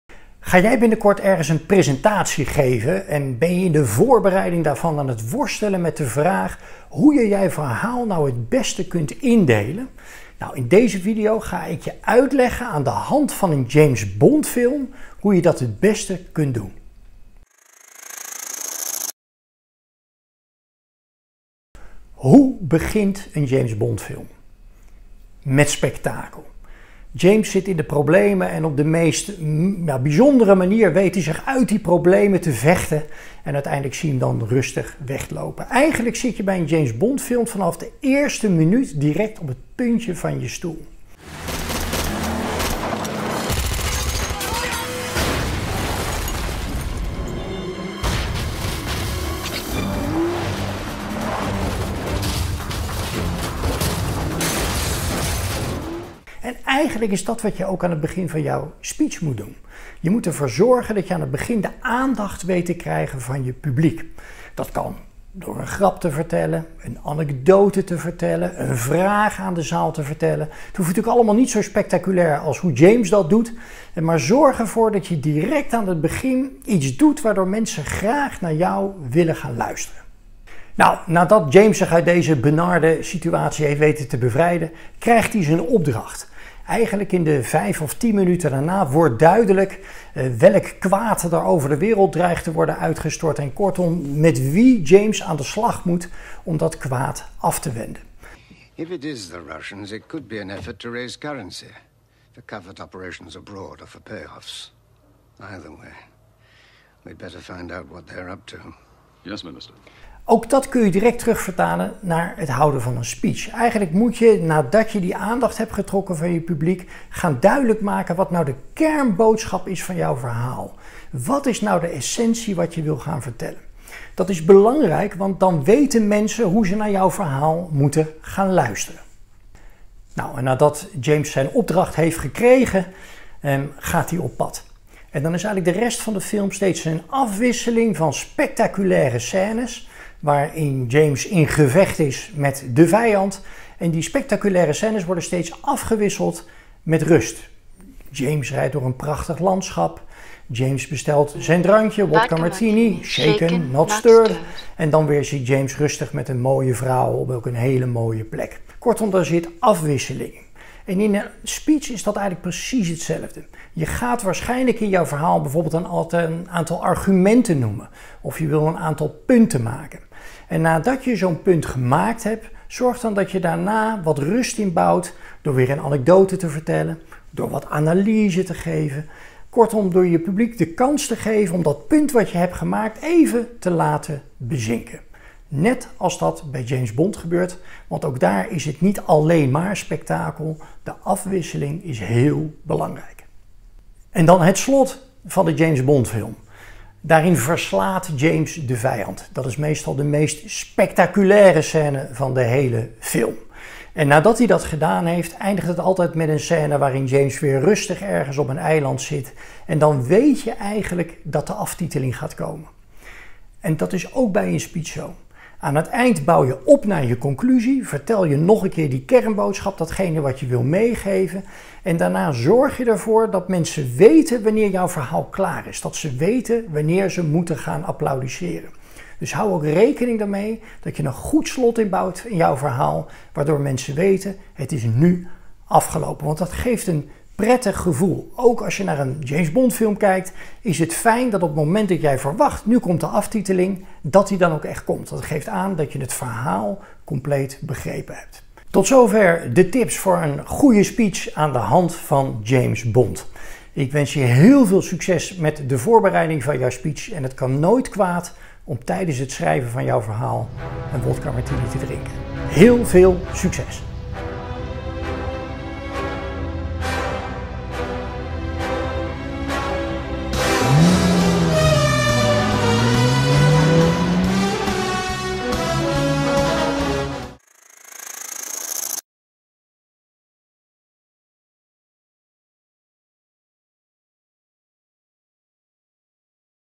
Moet jij speechen? Gebruik de James Bond-techniek | PRESENTEERTIP
De spanning en sensatie waar 007 zich in bevind lijken op het eerste gezicht misschien niets te maken te hebben met een goede speech. Toch weten de James Bond films mensen al jaren te boeien en te vermaken. Hoe dit voor jou voor een goede speech kan zorgen vertelt Roderik van Grieken in deze video.